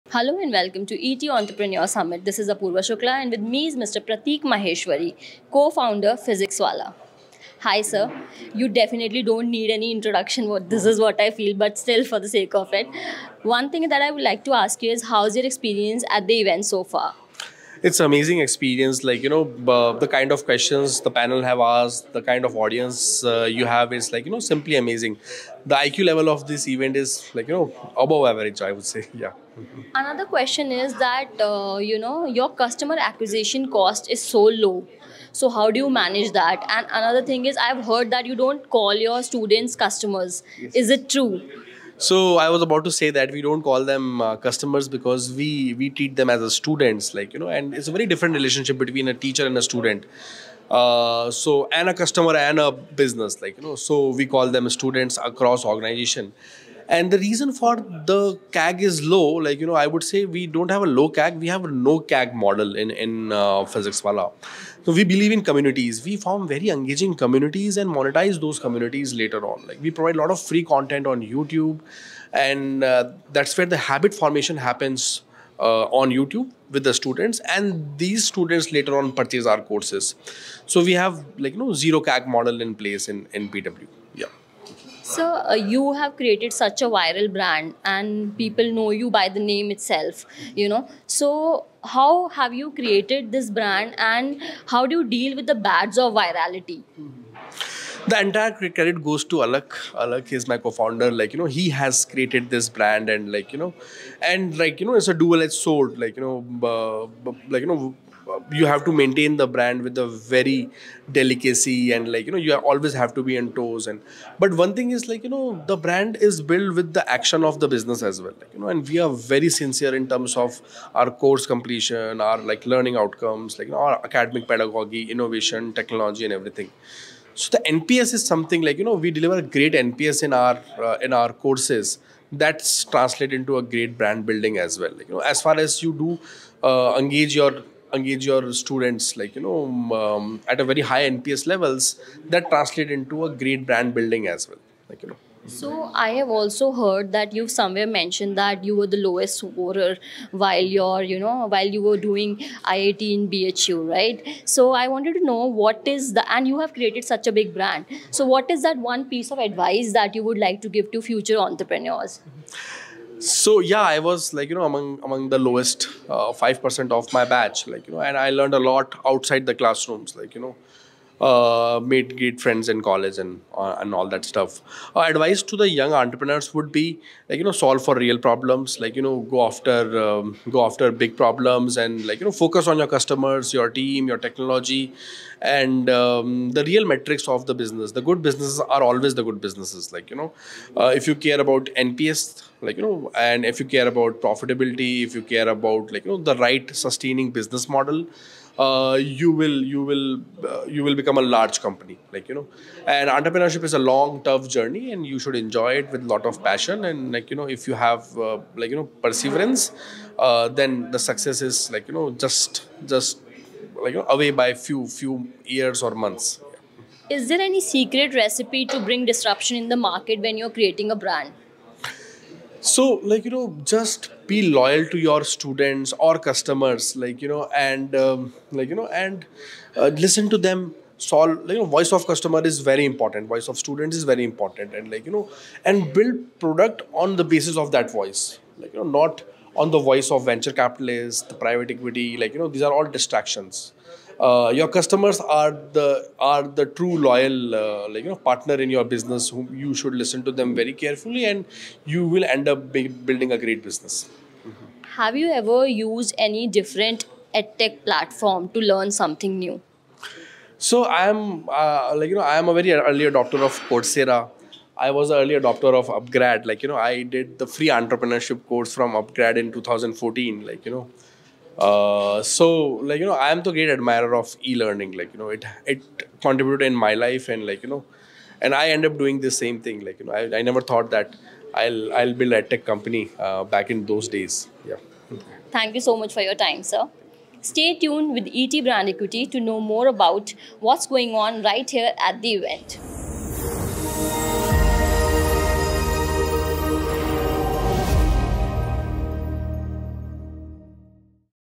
Prateek Maheshwari shares his transformative experiences at ETEntrepreneurSummit!
Unveiling the power of perspective! 🎤

Dive into ETEntrepreneurSummit, where luminaries share their transformative experiences and insights gained from the event.

Prateek Maheshwari, Co-founder, PhysicsWallah shares his experience and his advice to future entrepreneurs, and more, exclusively at ETEntrepreneurSummit!

Get ready to be inspired and empowered by their stories!

ETBrandEquity.com is the online destination of Brand Equity, India's premier source of information, news and opinion on marketing and advertising.